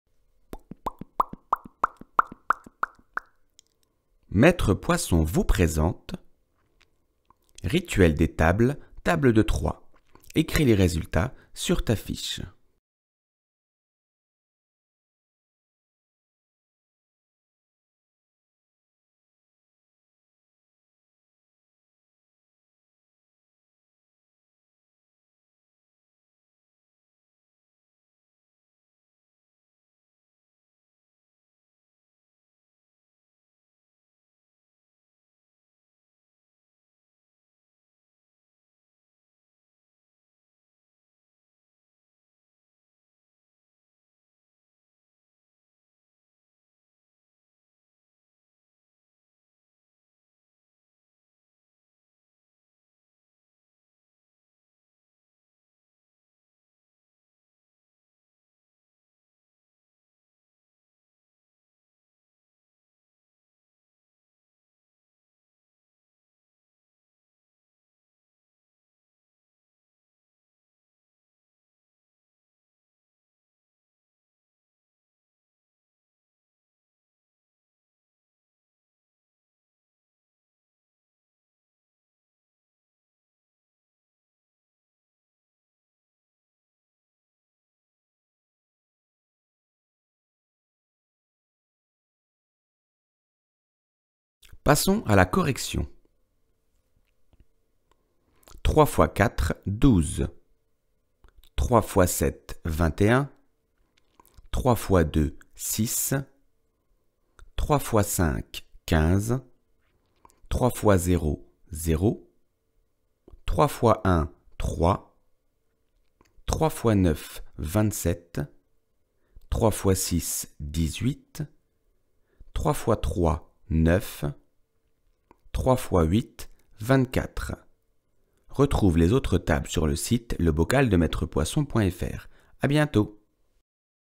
Table de multiplication X 3 - Apprendre à calculer
Voici la seconde vidéo d'une longue série de supports éducatifs pour apprendre les tables de multiplications.

Ces vidéos sont accompagnées par des fiches à télécharger et à imprimer gratuitement que vous pouvez retrouver sur le

Vous trouverez des ressources pédagogiques pour la maternelle, l'école élémentaire (cycle 2 et 3) mais également pour le collège.